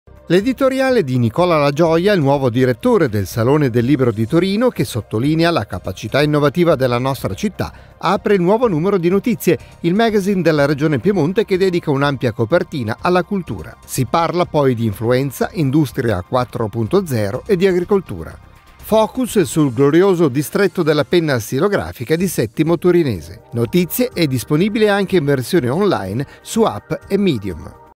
Il nuovo “Notizie”
L’editoriale di Nicola Lagioia, il nuovo direttore del Salone del Libro di Torino, che sottolinea la capacità innovativa della nostra città, apre il nuovo numero di Notizie, il magazine della Regione Piemonte, che dedica un’ampia copertina alla cultura. Si parla poi di influenza, industria 4.0 e di agricoltura. Focus sul glorioso distretto della penna stilografica di Settimo Torinese. Notizie è disponibile anche in versione online, su app e Medium.